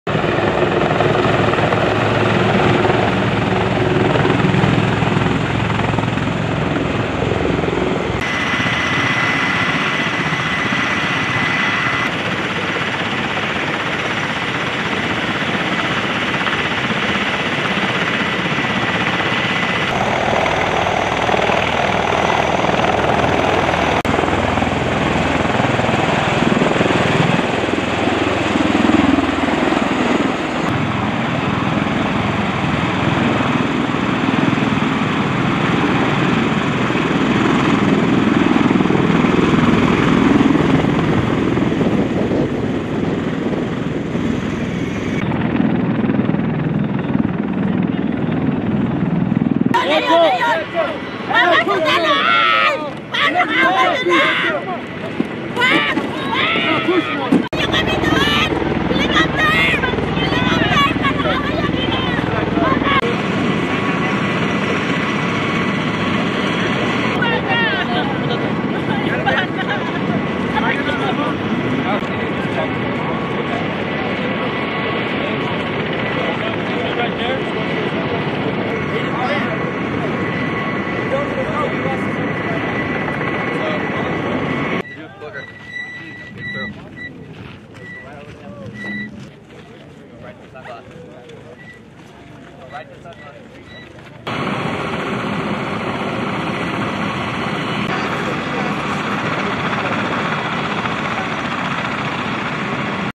Watch to See How 3/7 Handles an Evacuation Operation!
U.S. Marines with 3rd Battalion, 7th Marine Regiment, 1st Marine Division conducts a non-combatant evacuation operation (NEO) exercise, at part of Weapons and Tactics Instructor (WTI) course 2-23, at Marine Corps Air Ground Combat Center, Twentynine Palms, California, April 14, 2023.

3/7 conducts a non-combatant evacuation operation exercise
TWENTYNINE PALMS, CA, UNITED STATES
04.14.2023

 WTI is a seven-week course providing standardized advanced tactical training and certification of unit instructor qualifications to support Marine aviation training and readiness. (U.S. Marine Corps video by Lance Cpl. Jacquilyn Davis)

Film Credits: Video by Lance Cpl. Jacquilyn Davis
Marine Corps Air Ground Combat Center